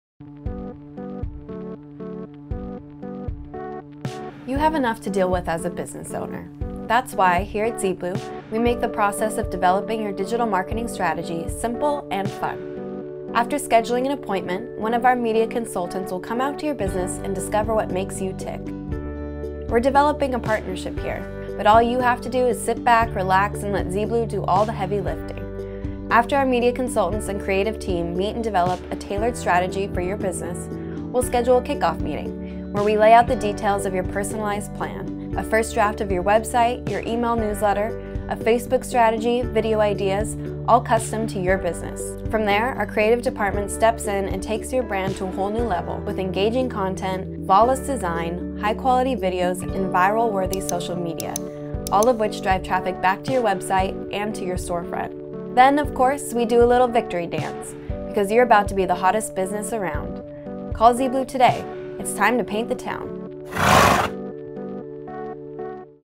ZeeBlu: Our Process of Using Marketing to Support Local Business
ZeeBlu is a digital marketing agency focused on supporting local business in Santa Barbara, CA.